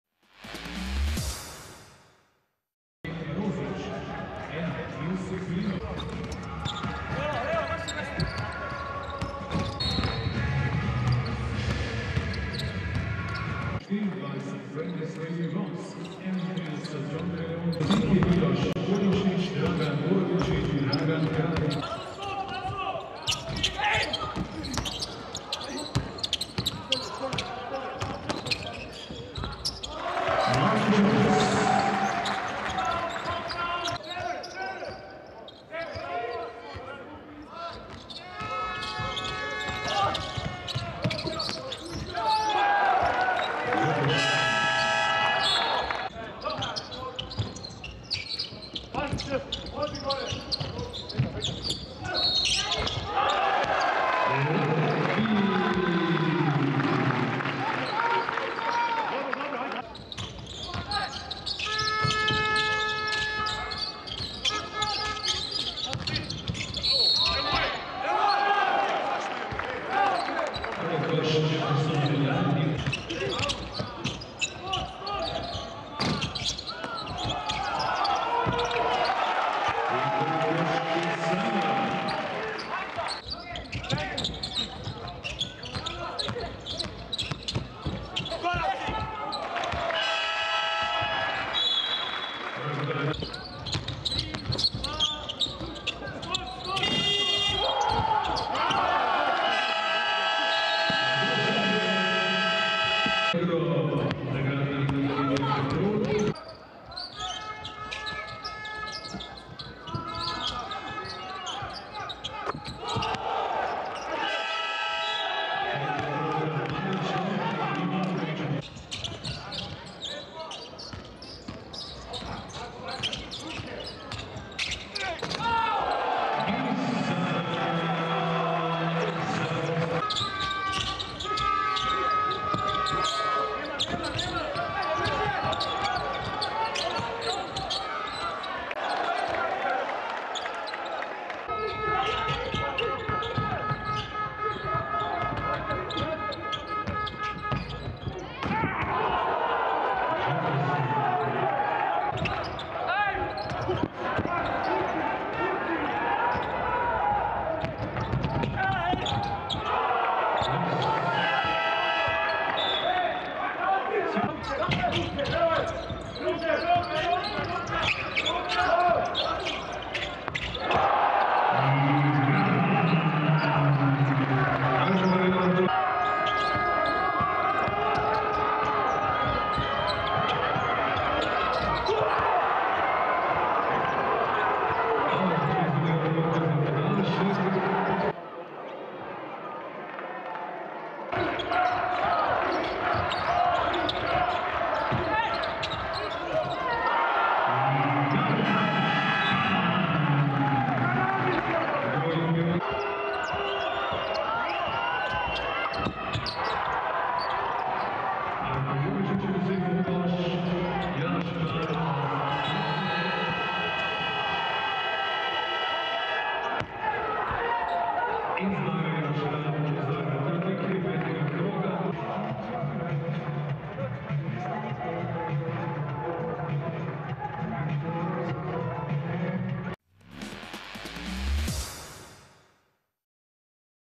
ABA Liga 2018/19 highlights, Round 5: Petrol Olimpija - Cibona (27.10.2018)
Take a look at the highlights of 2018/19 season ABA League game between Petrol Olimpija and Cibona in Stožice Arena, Ljubljana.